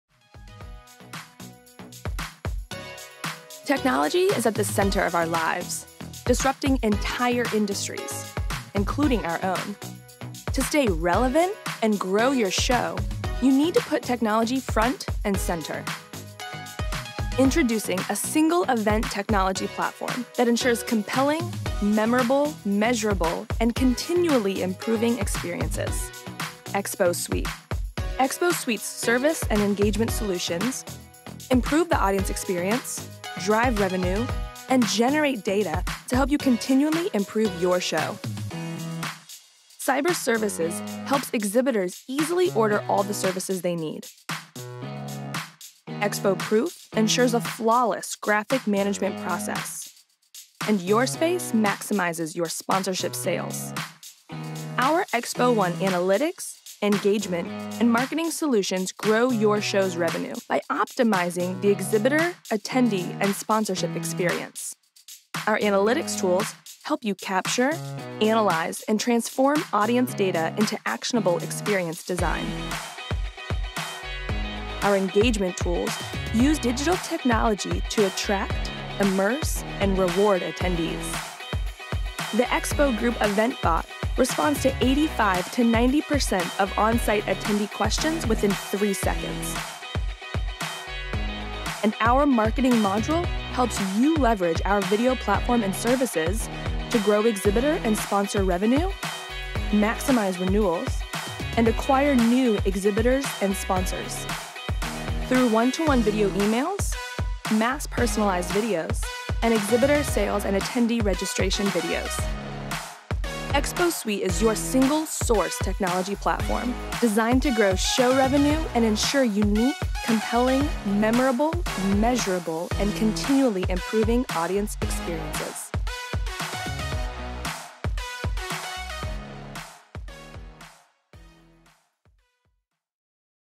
The Expo Group ExpoSuite™ from The Expo Group
Now, all the digital tools you need to improve show management and attendee engagement are in one, easy-to-use platform.